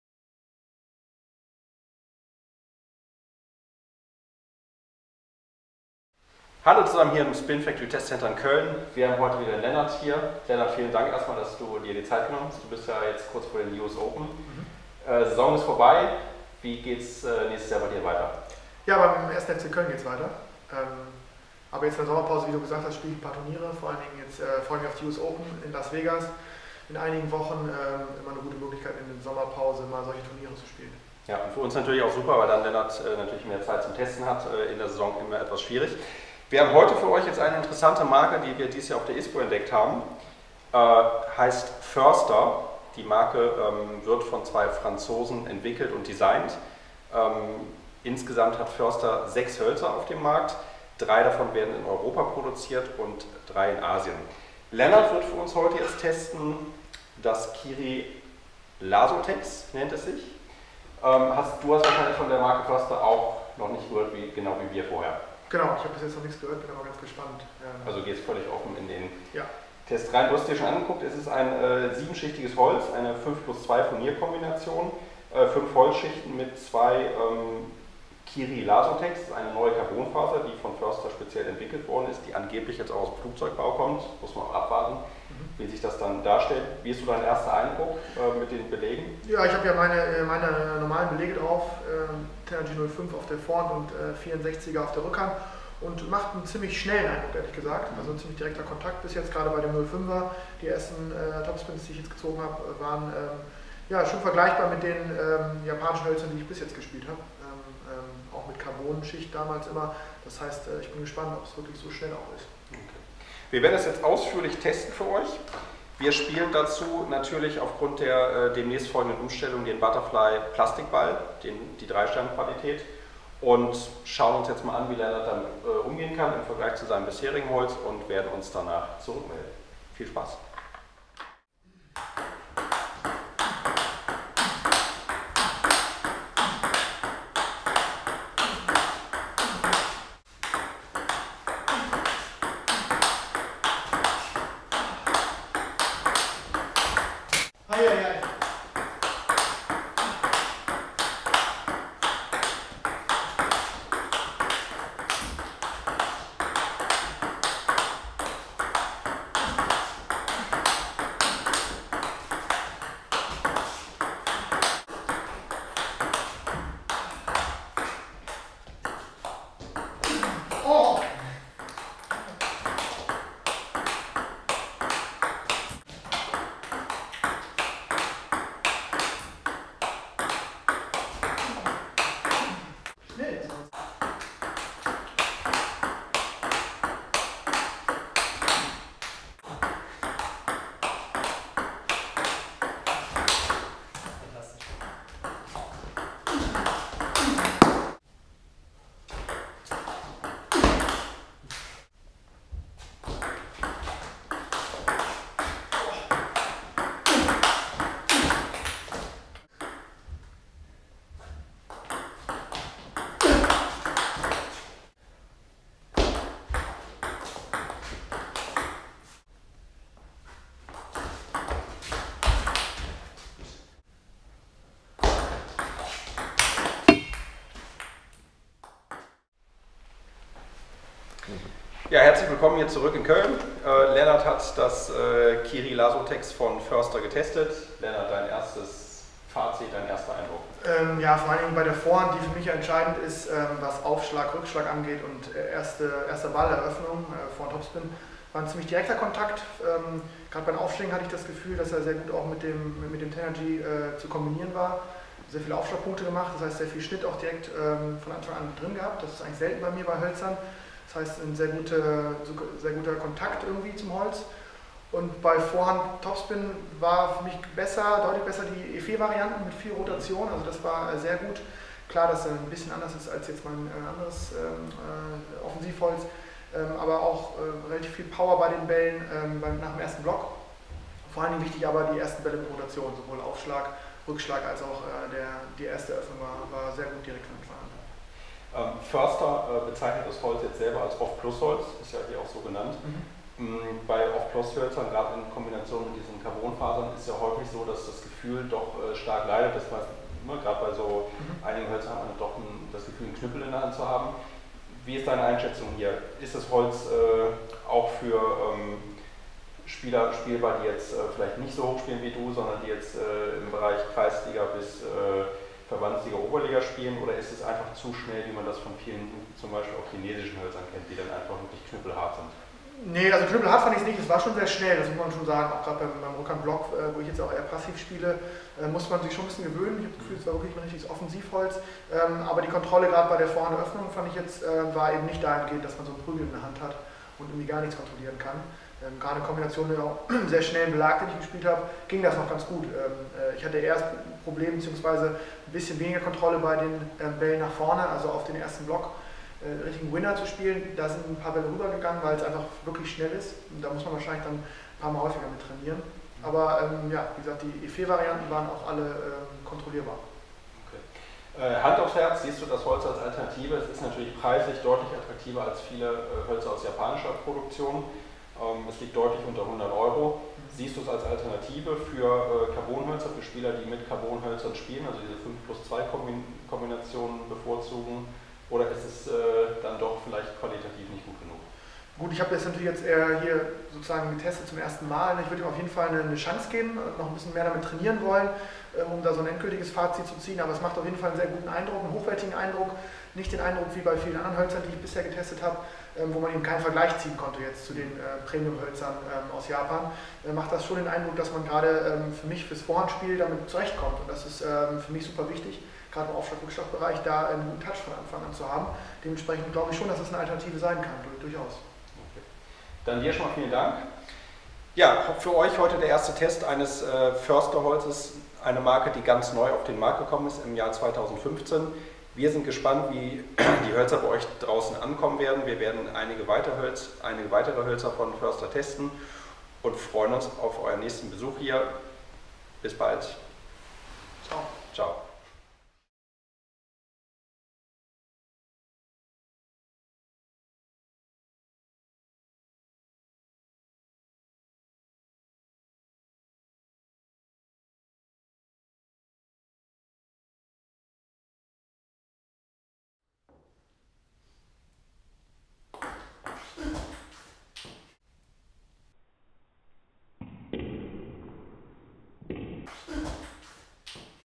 Förster - Kiri Lazotex: getestet von Lennart Wehking (10-07-15)
Lennart Wehking testet das Förster Holz Kiri Lazotex (OFF+) im spinfactory Testcenter. Weitere Informationen zum Produkt findet Ihr unter spinfactory.de.